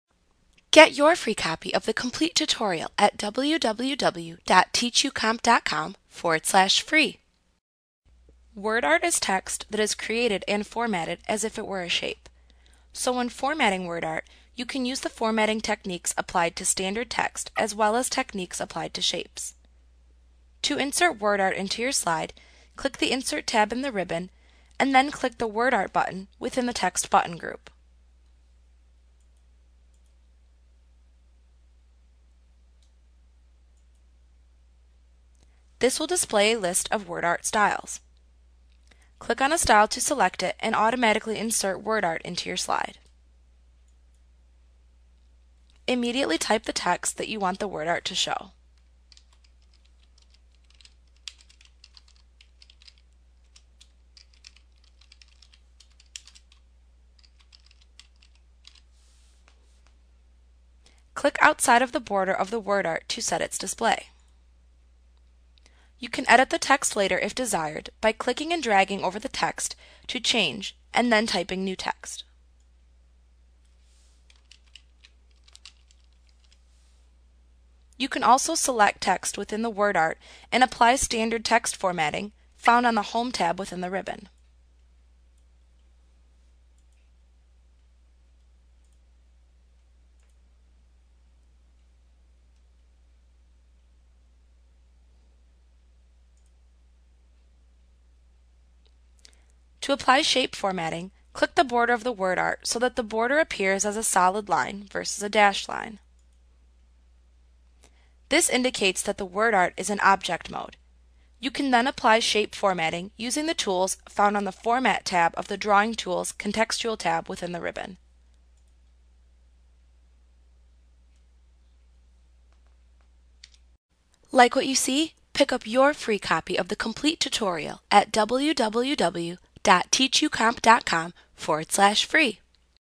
PowerPoint 2013 Tutorial Inserting WordArt-2013-2010 Microsoft Training Lesson 11.4
Learn how to insert wordart in Microsoft PowerPoint - the most comprehensive PowerPoint tutorial available. Visit us today!